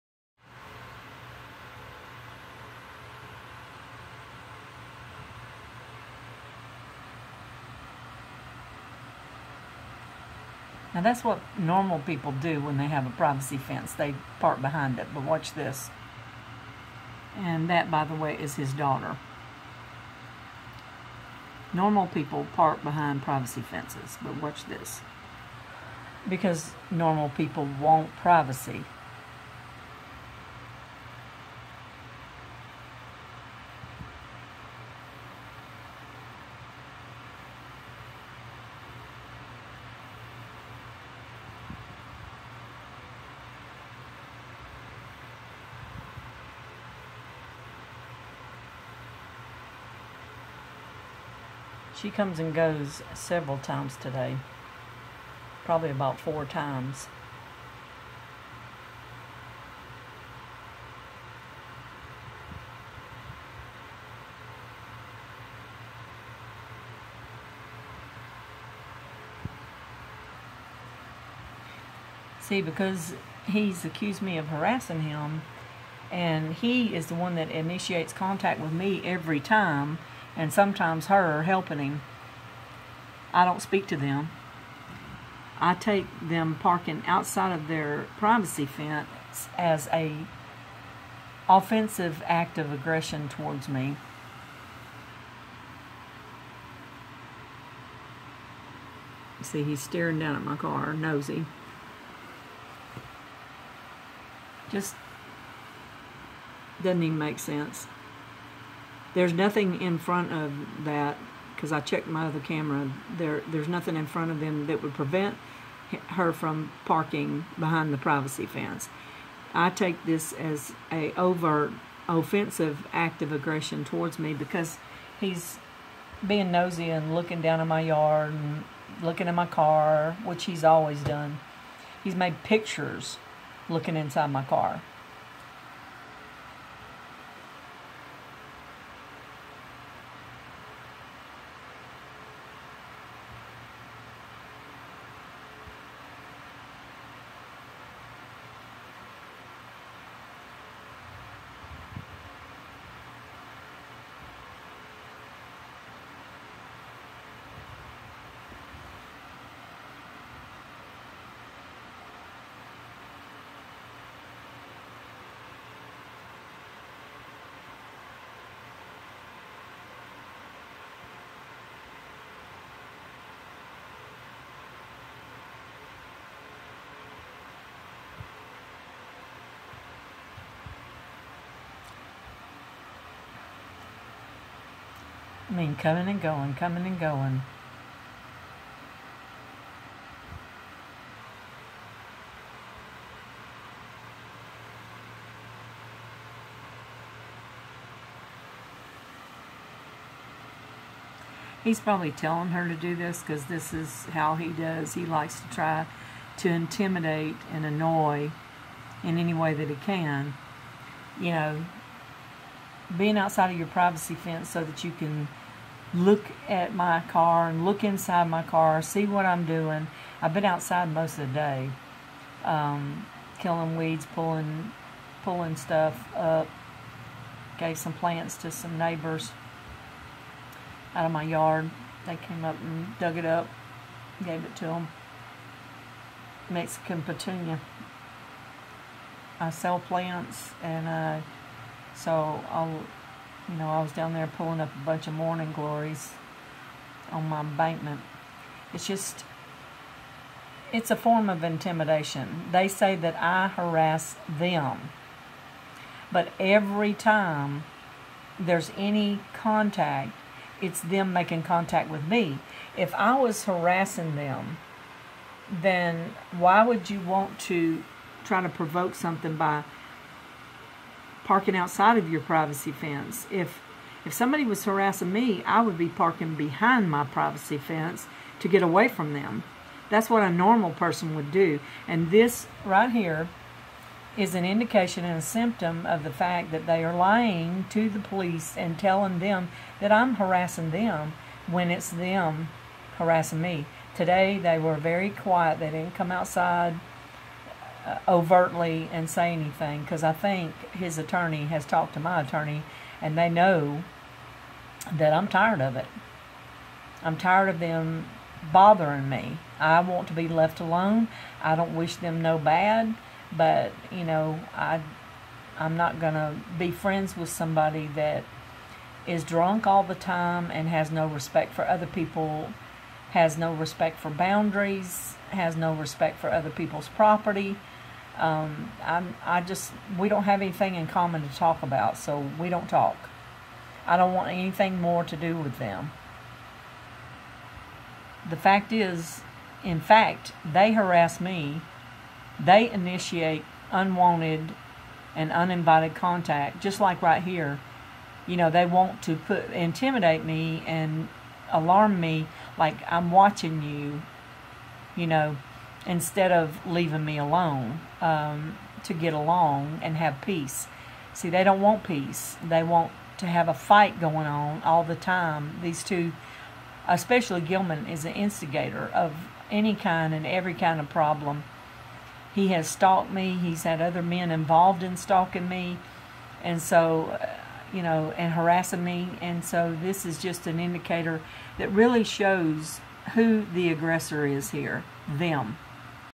06.25.2019 - * * # ⛌ Neighbor Harassing and Stalking Me
Brandy McKinney Bryant, Gilmans daughter helping him to harass, intimidate and annoy. Now this may seem like a trivial point, but if a person was being harassed as he claims, a normal person would park behind their "privacy" fence to avoid the person. So parking outside of his privacy fence, I take it as a subtle sign of agression towards me. Bc it appears to me he is doing this to annoy, intimidate and alarm, all elements of not just harassment, but stalking. I am down on the other side of my yard down an embankment, pulling and spraying weeds. I am unaware of him looking inside my car and staring at me with his typical narcissistic psychopathic, sociopathic death stare he gives.